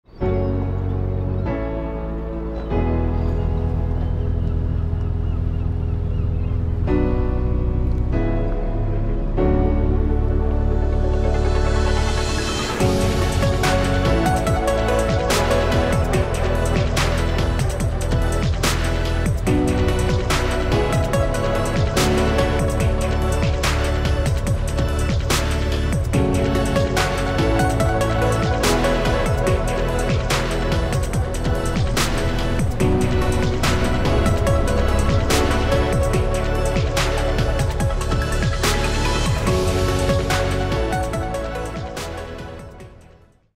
Mitchell Park & Marina, Greenport, New York
Aerial video of Mitchell Park Marina is located in the center of the waterfront Village of Greenport.